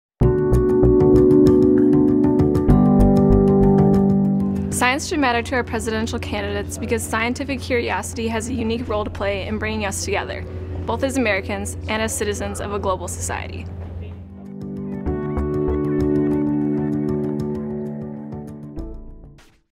Cassandra Sundaram, University of Notre Dame
Throughout the 2016 presidential campaign, The Science Coalition is asking people to answer the question: Why should science matter to the presidential candidates?